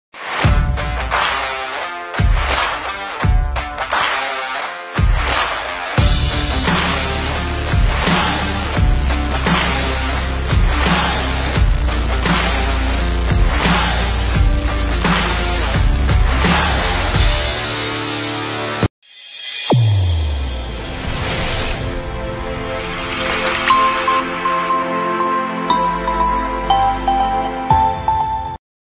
Bringing a taste of luxury, performance, and sophistication like never before with New
Bringing a taste of luxury, performance, and sophistication like never before, here’s a glimpse of some of the New Safari’s class leading features. Bookings Open.